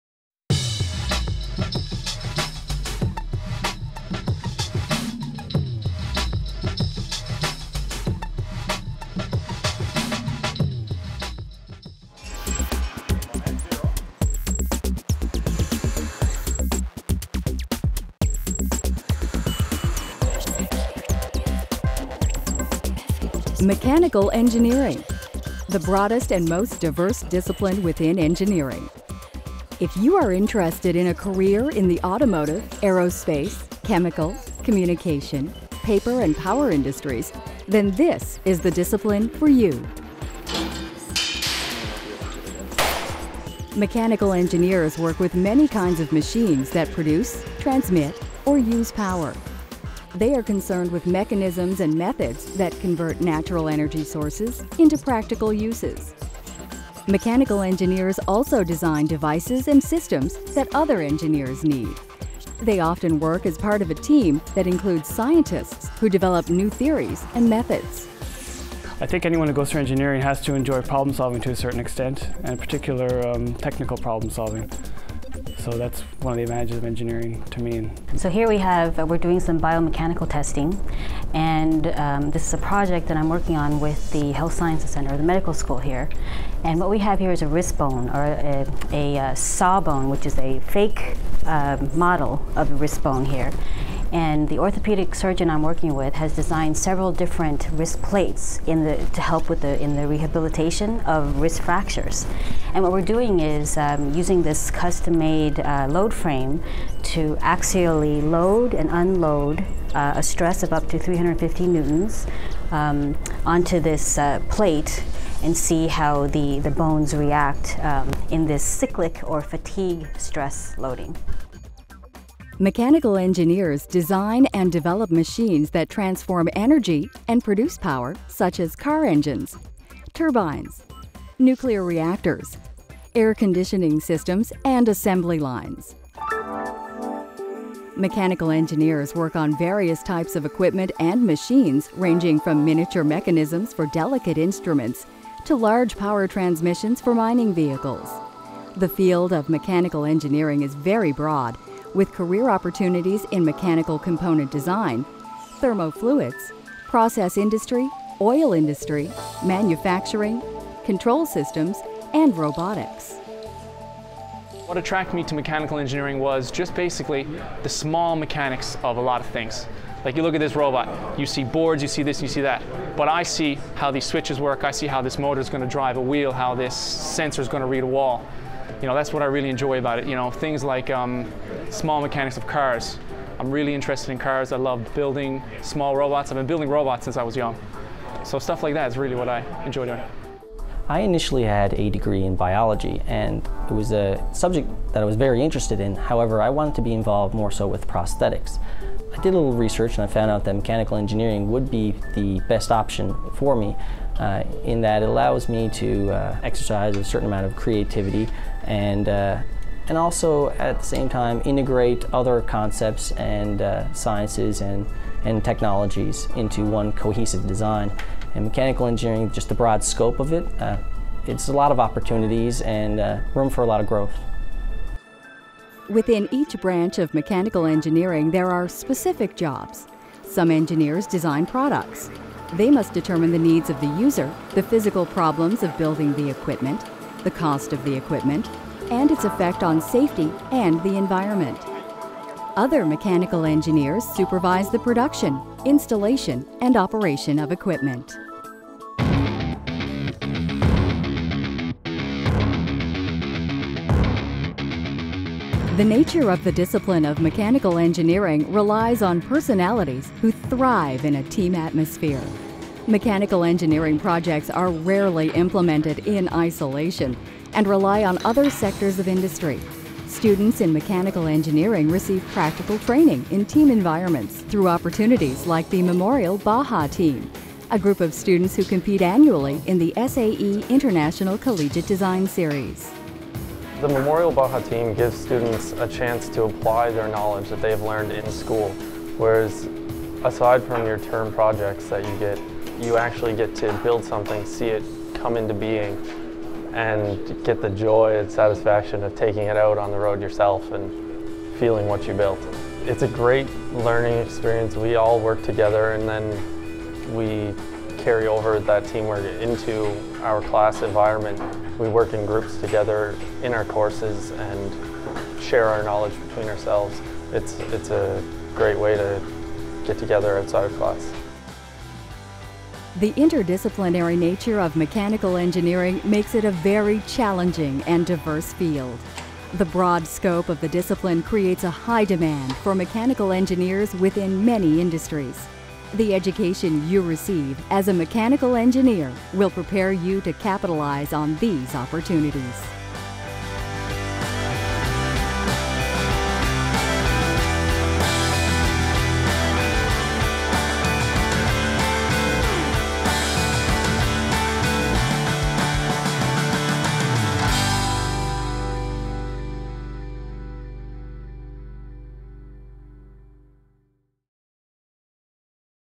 Mechanical Engineering at Memorial University
As a part of the Mechanical Engineering program at Memorial University, students study courses in mechanics, solid mechanics, vibration, controls, design, fluid mechanics, thermodynamics, heat transfer, mathematics, materials, mechanical systems and basic electronics.